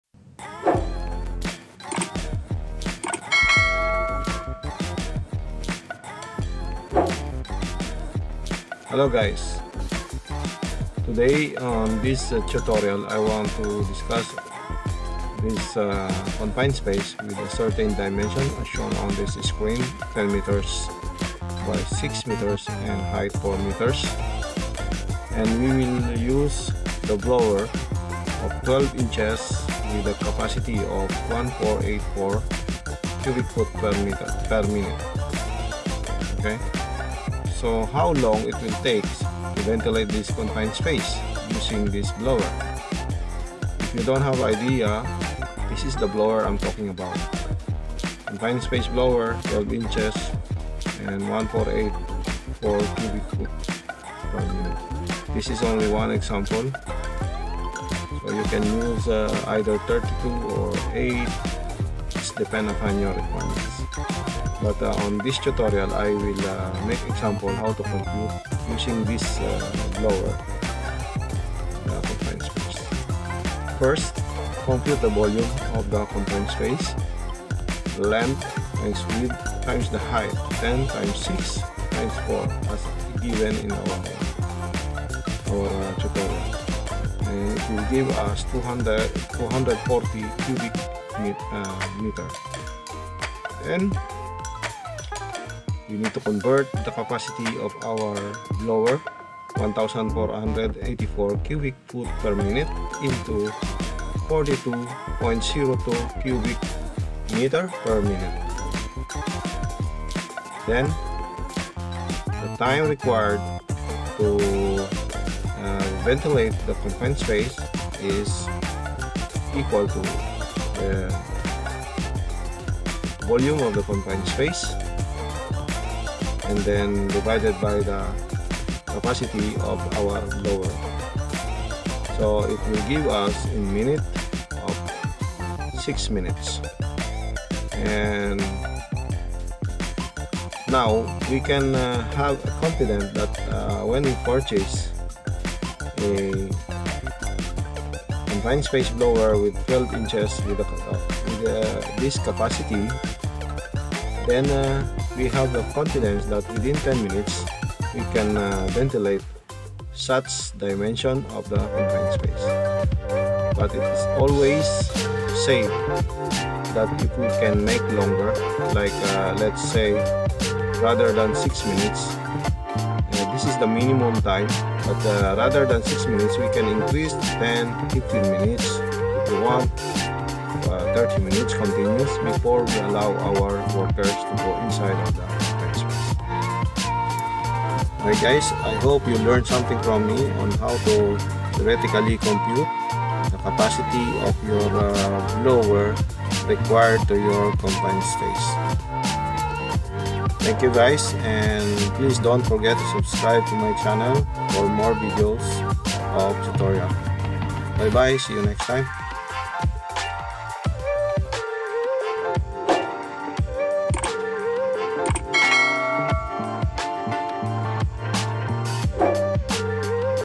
Ventilating Confined Space
Hi Friends,
This video tutorial discussed how to estimate ventilating confined space and make idea what capacity of blower required to a particular size of confined space.

Cheers...